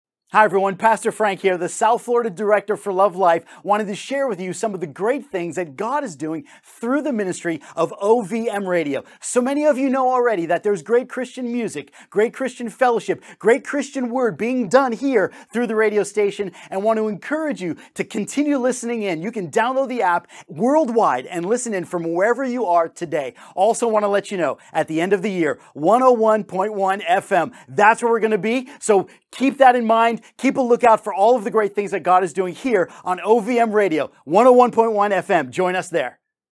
OVM Radio Expanding to 101.1 FM – Join Us on the Journey!
In this video, Pastor Frank Trotta, the South Florida Director for Love Life, shares exciting updates about the ministry of OVM Radio. He highlights the incredible work God is doing through this Christian radio station, which features inspiring music, fellowship, and powerful biblical teaching. He also announces the station's upcoming move to 101.1 FM by the end of the year, inviting everyone to join in and experience the continued blessings and ministry that OVM Radio offers. Tune in and be part of this incredible journey!